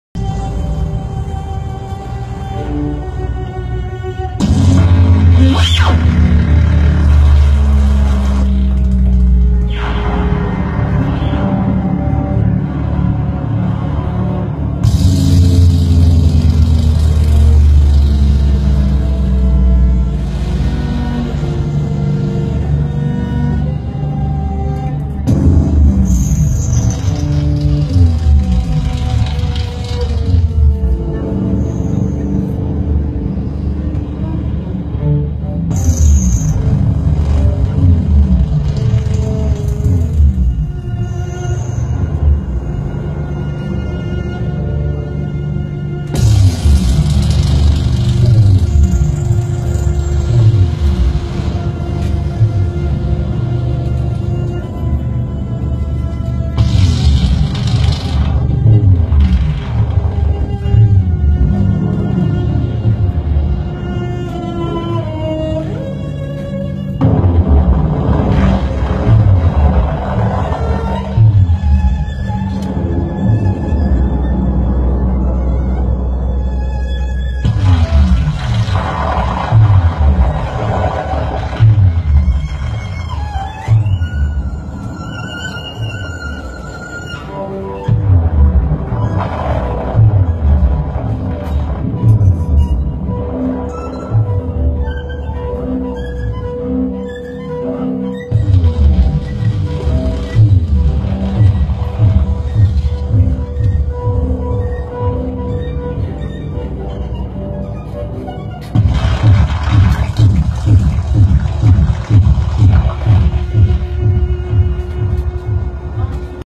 Sound Electric & Cello By Koichi Shimizu & Yui Saowakhon
Live At Studio lam,  Bangkok
Feb.2018

ติดตามผลงานและกิจกรรมของพี่ยุ้ยเพิ่มเติมตามลิ้งด้านล่างนะคะ